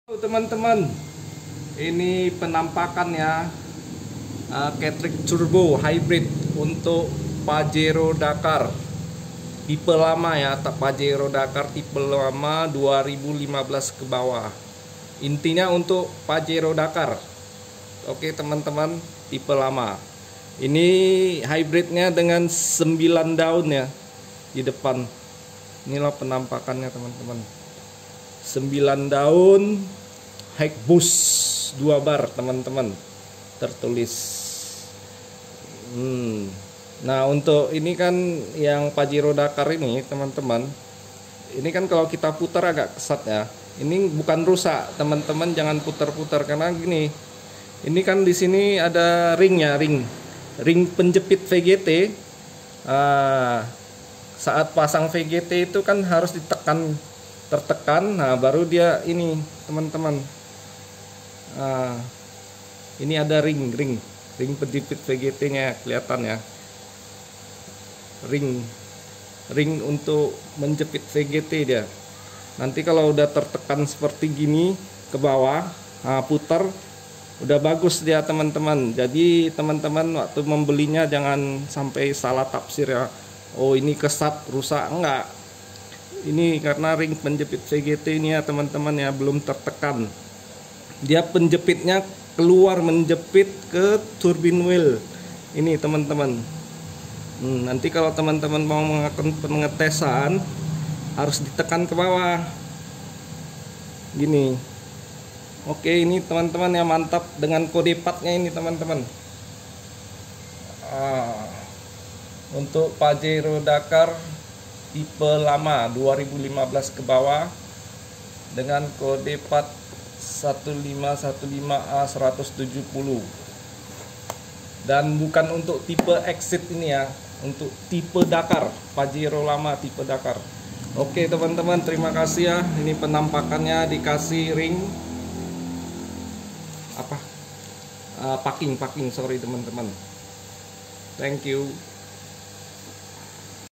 CATRIDGE TURBO PAJERO DAKAR HYBRID 4D56 Tahun 2015 Kebawah, Plug n Play!!
Halo teman-teman, sudah lama Pajero Stan tidak upload video, nih. Kali ini kita akan mulai dari upload video yang ringan seperti videi kali ini.

Video kali ini akan membahas tentang catridge turbo untuk jenis mobil pajero dakar tahun 2015 kebawah. Yuk langsung aja ditonton!

Bagi yang berminat belanja catridge langsung saja ke tokopedia kita, ya